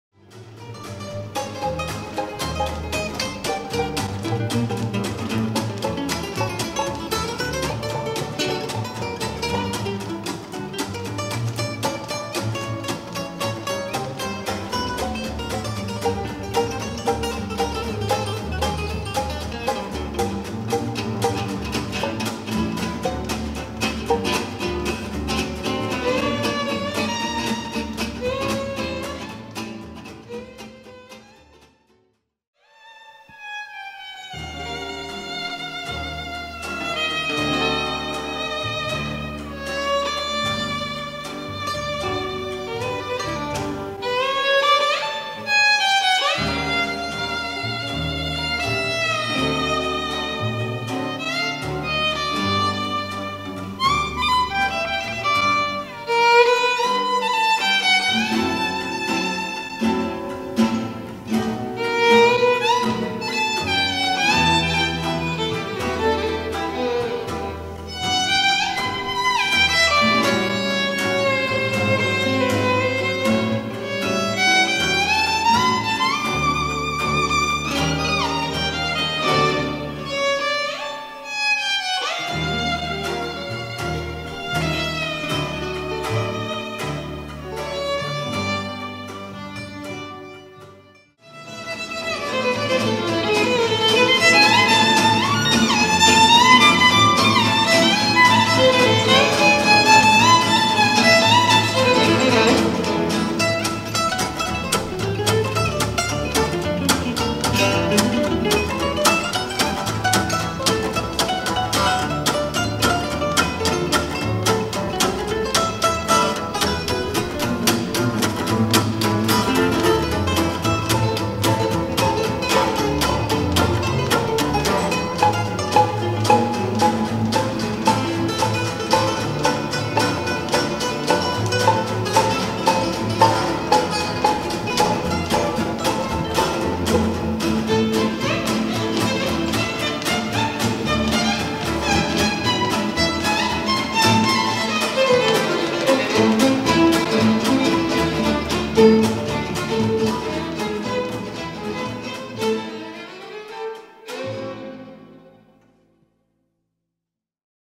Latvian Manouche quartet
Aleksejs Bahirs - vijole
Gatis Ziema - ģitāra
Ģirts Pavēnis -  ģitāra
Ginta Garūta - kontrabass